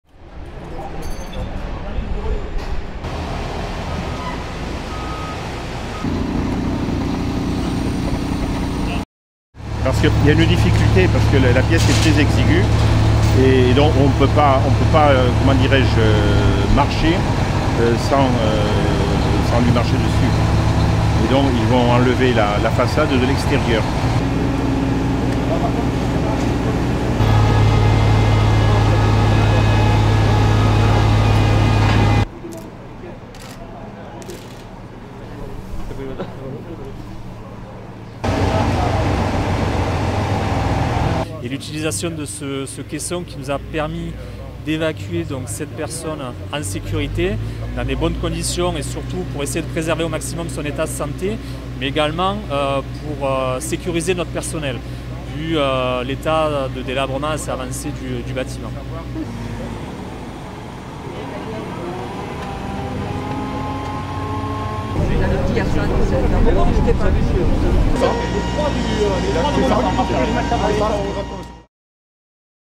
Evacuation réussie d'un homme de 300 kg coincé chez lui à Perpignan | AFP
Une opération aussi délicate qu'inhabituelle à Perpignan : l'évacuation par les airs d'un quinquagénaire d'environ 300 kg coincé depuis des mois dans son appartement.